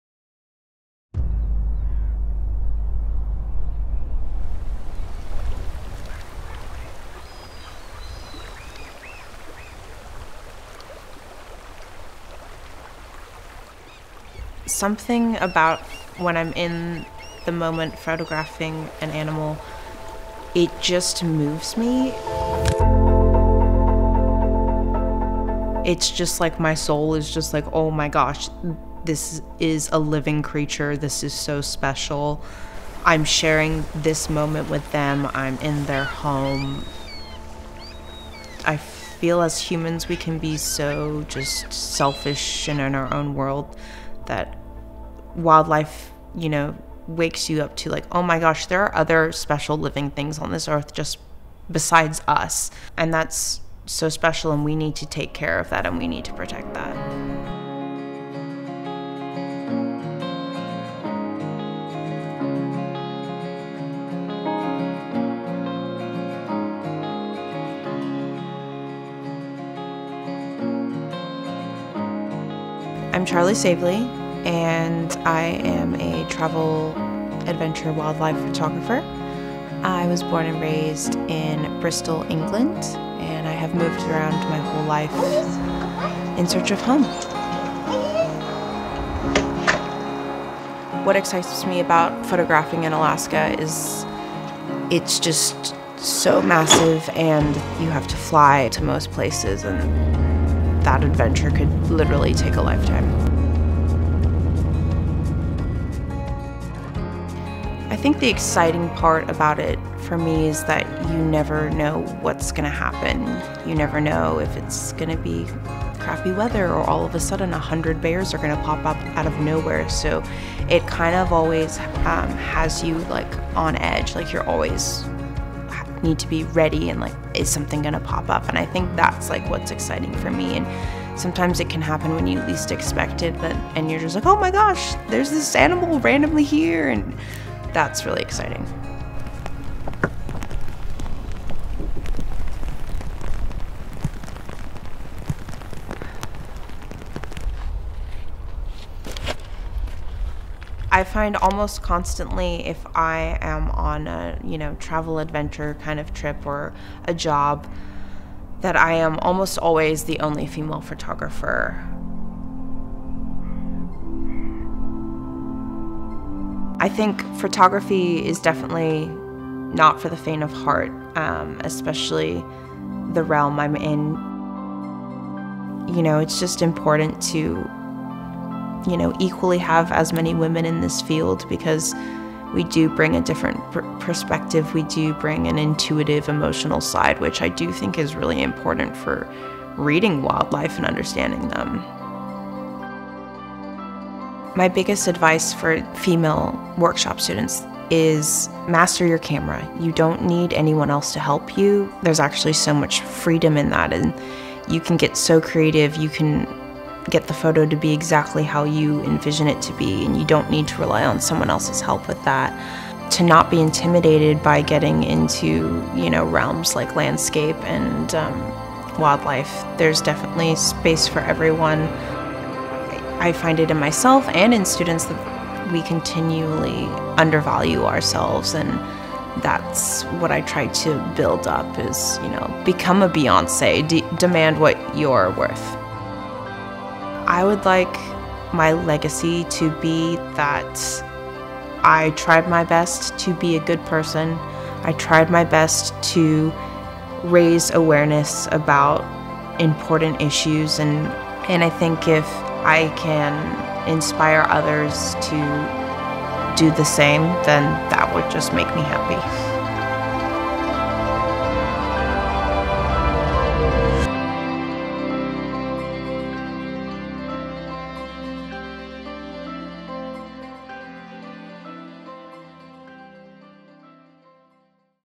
In Search of the Wild
A short film exploring what the wild means to Charly Savely.